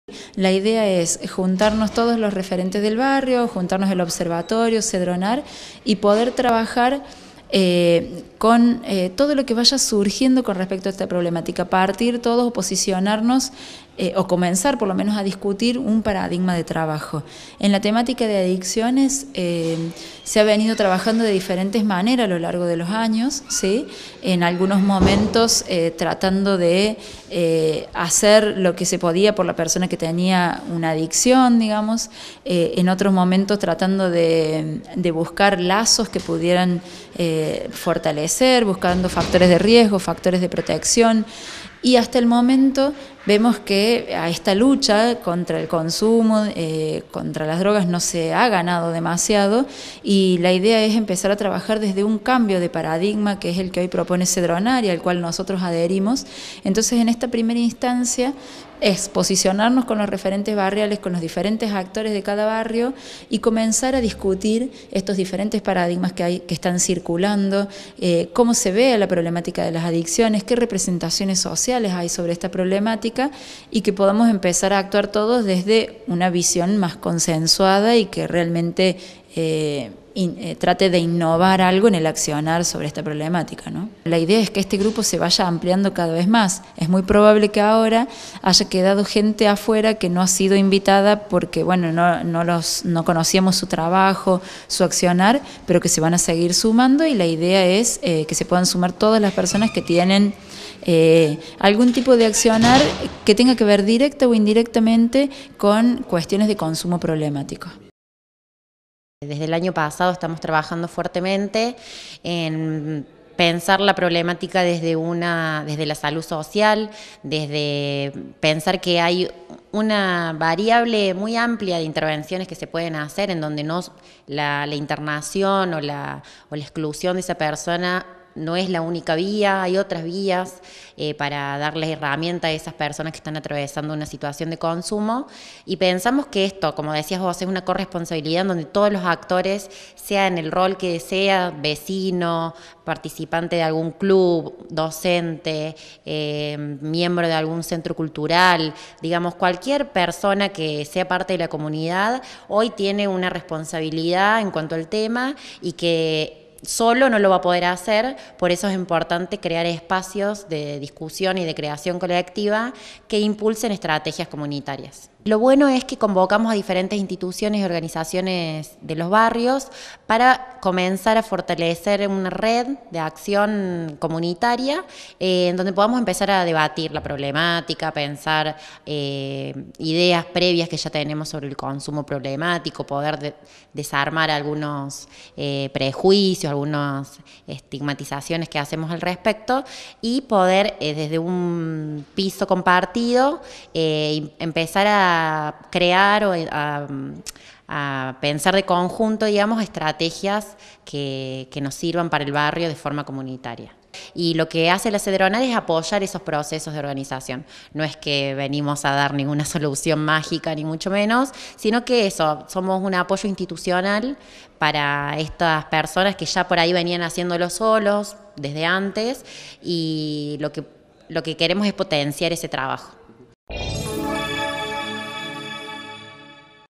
PUNTOS DE ENCUENTRO COMUNITARIOS
REALIZARON TALLERES EN ALBERDI Y BANDA NORTE